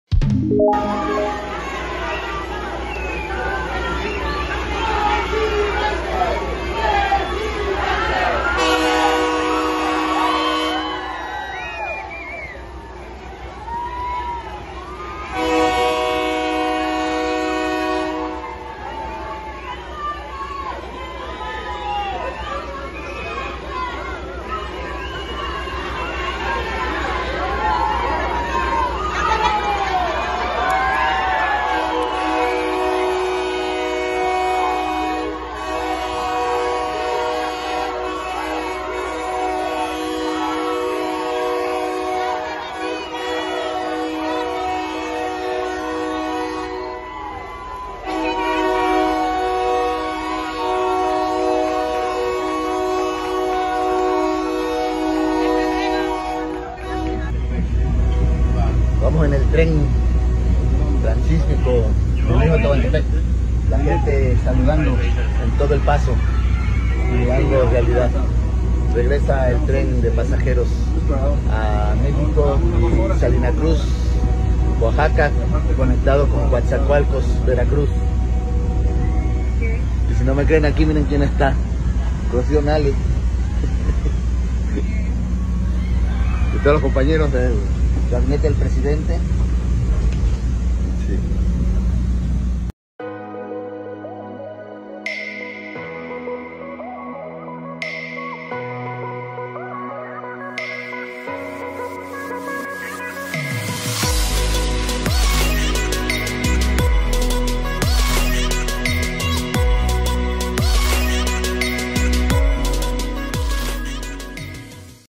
AMLO se sube al Tren Interoceánico en el Istmo de Tehuantepec.
La derecha debe estar que no la calienta ni el sol. AMLO realiza el primer recorrido de prueba a bordo del Tren Interoceánico en el tramo que va de Salina Cruz, Oaxaca, a Coatzacoalcos, Veraxcruz. Regresa el Tren de Pasajeros a México.